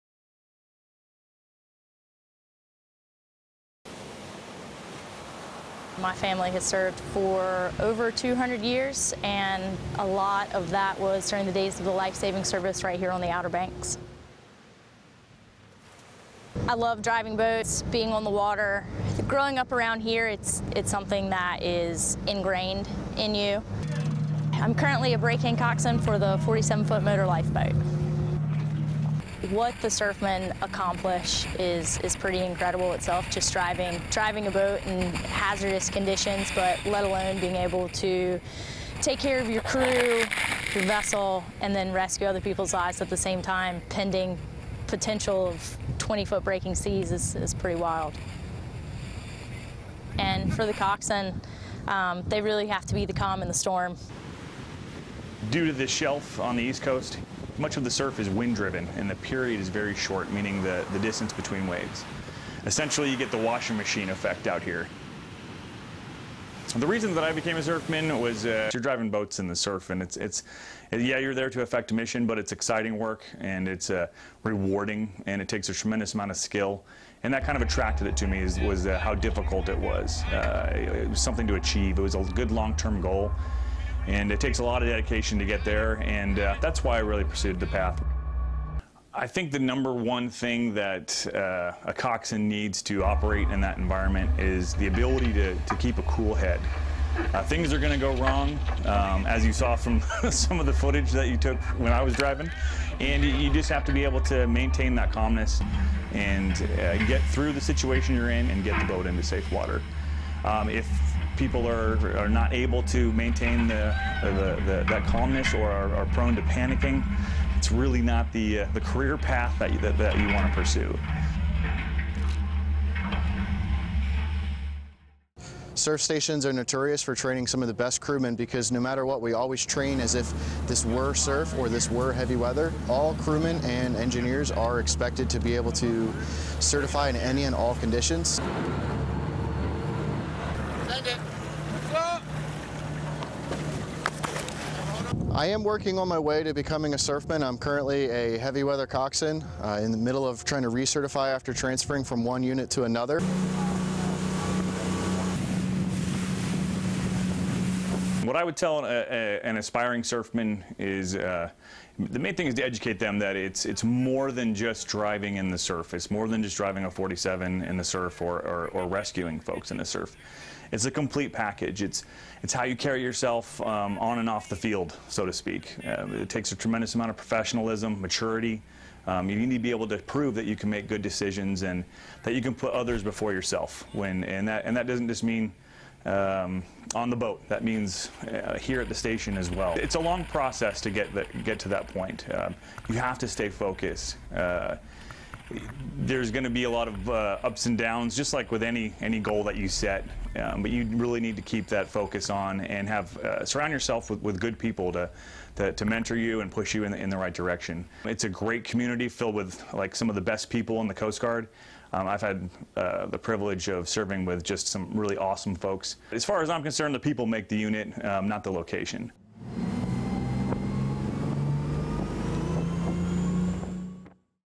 Coast Guard Surf Life OBX
Credit: Petty Officer 2nd Class Nate Littlejohn | Date Taken: 01/04/2016
Coast Guard 47-foot Motor Lifeboat crewmembers and operators train on North Carolina’s Outer Banks during January 2016. Surfmen and their crews operate in some of the harshest maritime environments the world has to offer. U.S. Coast Guard video by Petty Officer 2nd Class Nate Littlejohn, Station Oregon Inlet and Capt. Joseph Dufresne